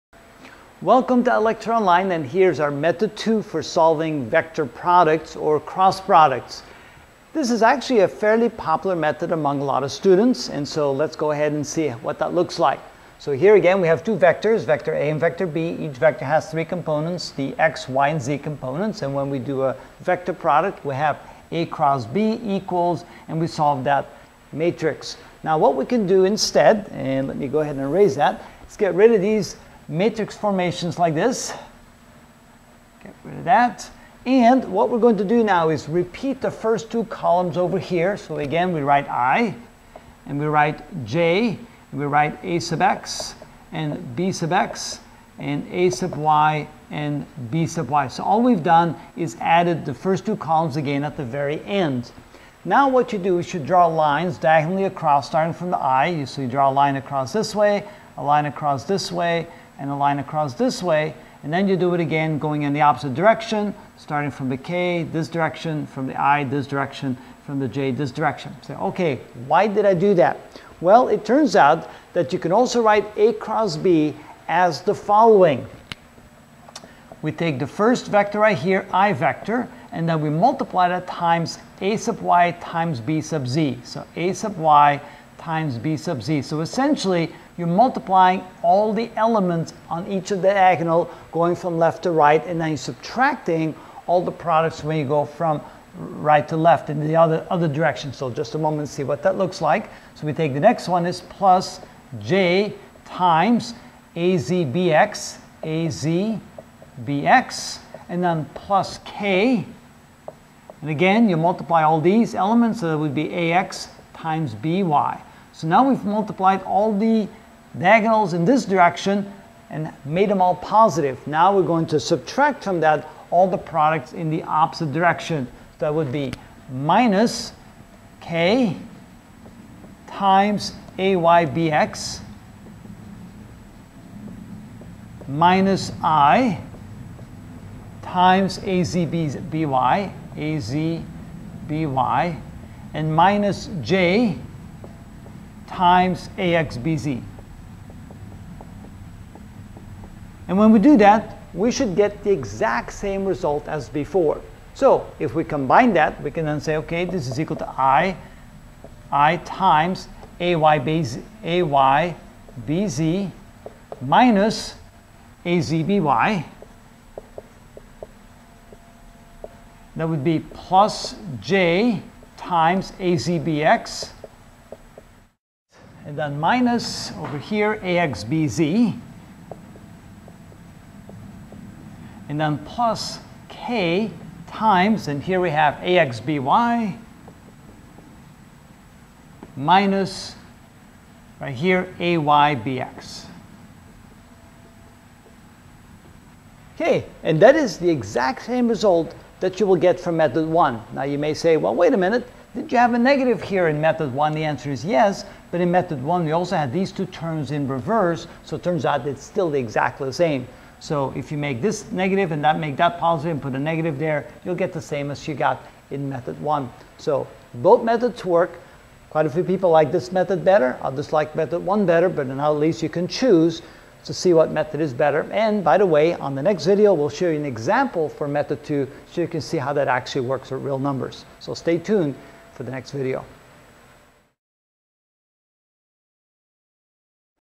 Mechanical Engineering: Rigid Bodies & Sys of Forces (8 of 47) Solving Vector Products: Method 2
In this video I will execute, in general, (method 2) vector product.